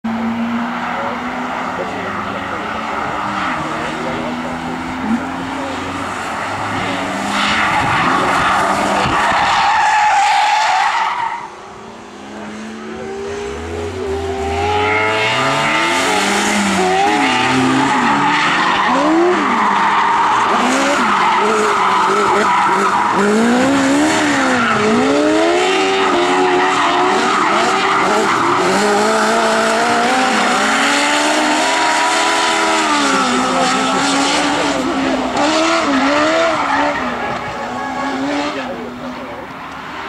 Drift Bordeaux Merignac circuit auto GTRS
Quand la Laser Mobile Turbo ne veux pas marcher alors il ne reste plus qu'a filmer les collègues drifteur ;-)
Julien 7€ pour le cone . et petite balade des buggy radiocommandé
15 Aout 2012